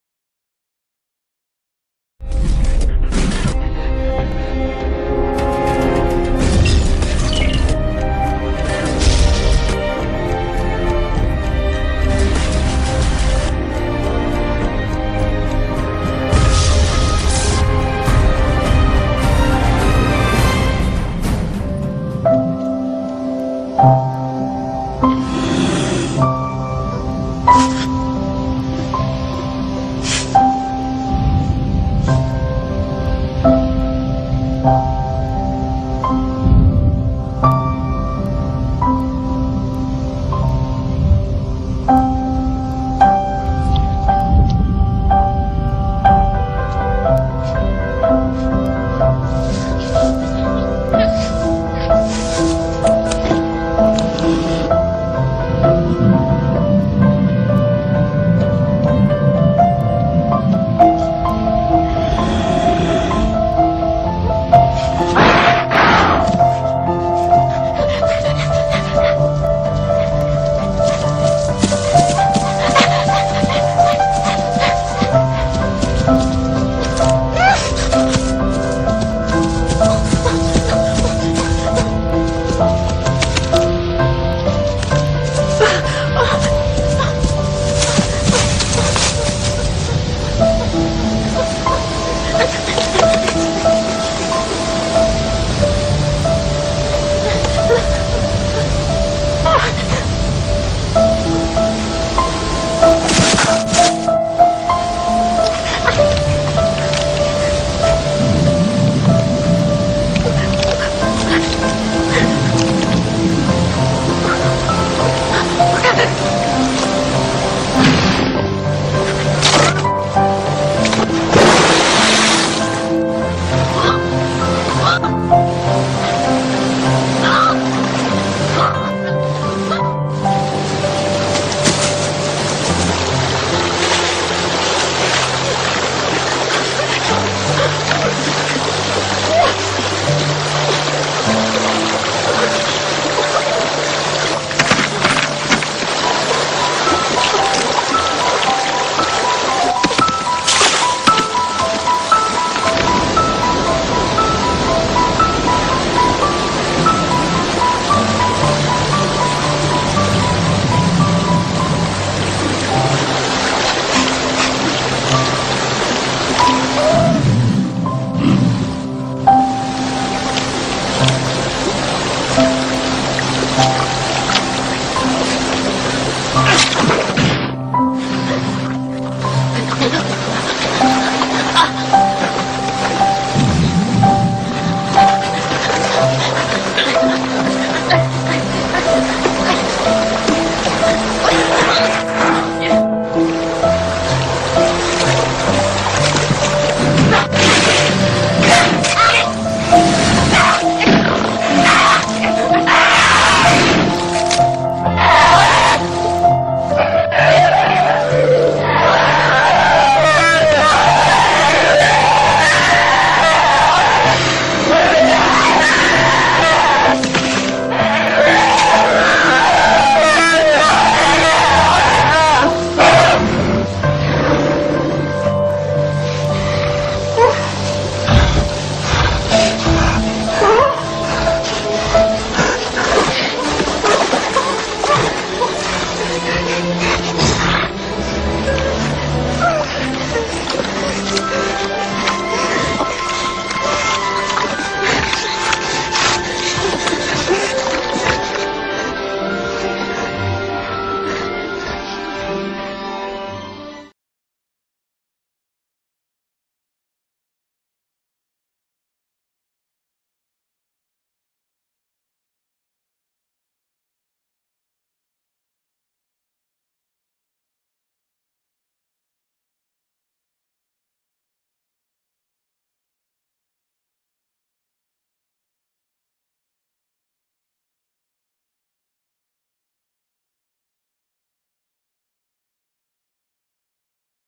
Reflexiónes
Un puma, un osezno, persecución al límite, una rama como única salida, caída al río, más persecución del depredador que no se da por vencido, el puma atrapa al osezno, zarpas y sangre, tensión, y finalmente, un oso mayor protege a la cría, amenaza al puma que huye, fundiéndose los osos en un abrazo de agradecimiento y vida.